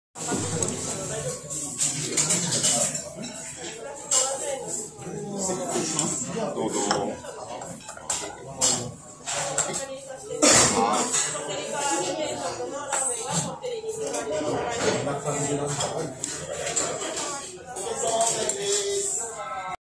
天下一品のこってりラーメンとコーンの相性は？こってり（コーントッピング）実はこってりとコーンの相性は抜群なのです。「明日もお待ちしています」
天下一品の
こってりラーメンと
コーンの相性は？

こってり（コーントッピング）
実はこってりとコーンの
相性は抜群なのです。
「明日もお待ちしています」